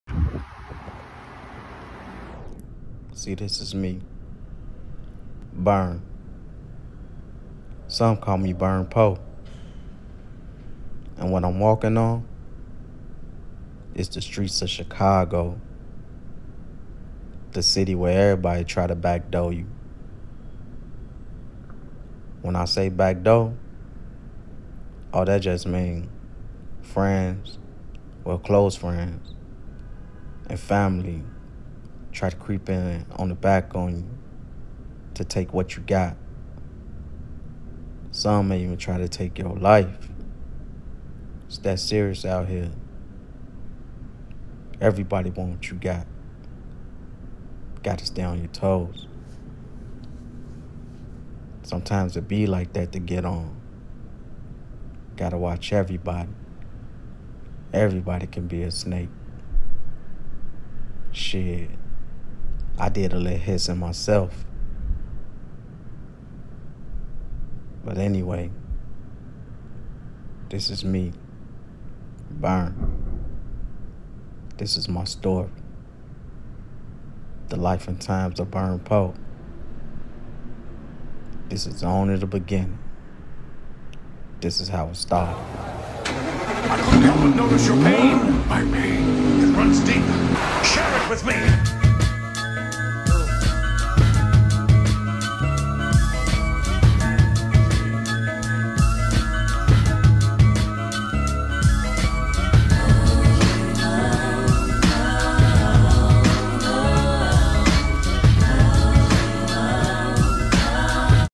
Life and times of Burn Poe… The intro
This series is about a young guy in the streets of Chicago. Who has been through everything you could possibly imagine… Through trails and tribulations he gives you his life story… Please watch and take a look at what he’s experienced… This is the Life and times of Burn Poe…